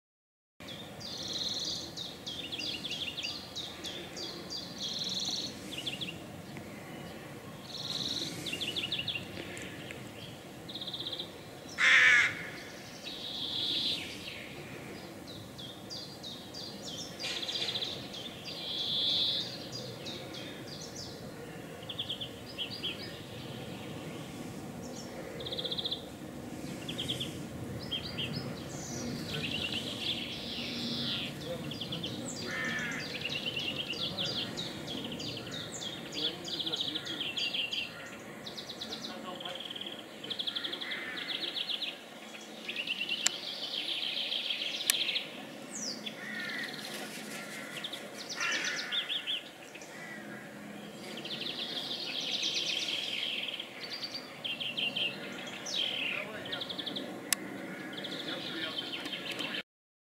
Вербейник
Вербейник обыкновенный - многолетнее травянистое растение с прямостоячими стеблями. Вербейник зацветает  в середине-конце июня,цветение длится около двух месяцев.